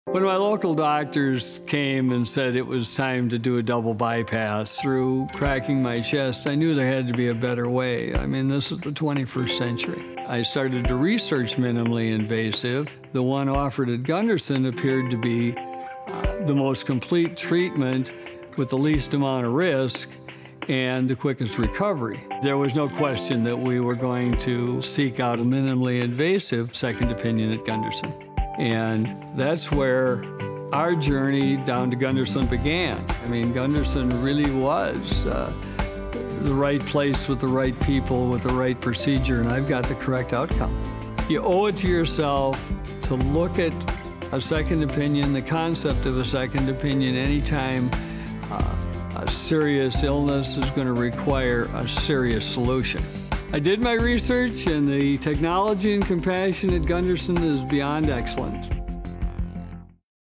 Don McDonald, Heart Surgery Pioneer TV spot
About 10 years ago Don suffered a heart attack. He was treated with angioplasty and stents to open blocked arteries. By December 2011, Don was in the emergency room yet again. This time the heart surgeon recommended bypass surgery. As Don did his research, the minimally invasive coronary artery bypass grafting or MICS-CABG technique used at Gundersen Lutheran appealed most to him.